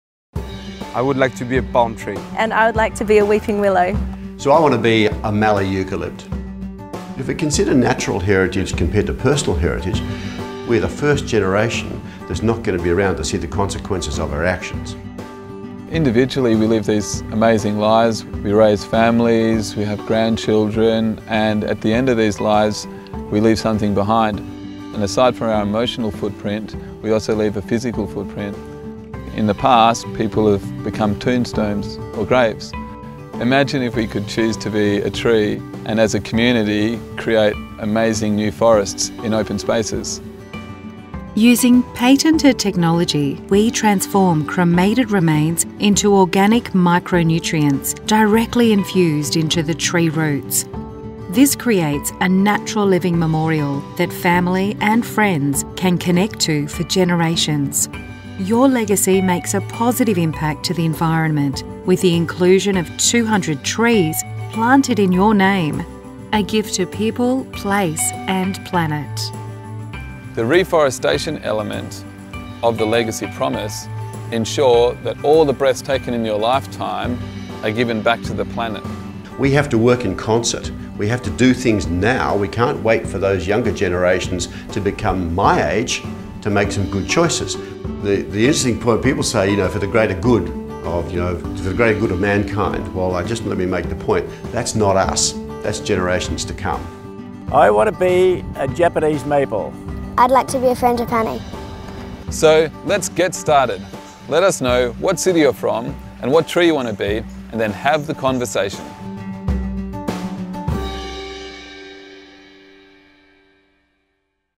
Living Legacy Forest- Living Memorial Trees
Living Legacy Forest has patented the world first organic science that turns cremated remains into nutrient molecules like nitrogen that actually help trees grow.

Few people know that cremated remains have the same pH as bleach and oven cleaner. Simply burying them even with biodegradable urns does not treat the ashes so they can become part of the tree and this is essential to restore and become a part of the circle of life with the soil biology.

With Living Legacy Trees your ashes restore the circle of life starting witht the soil biology. The means your ashes help the tree grow and continue to have an impact beyond the trees life through its seeds and seedlings.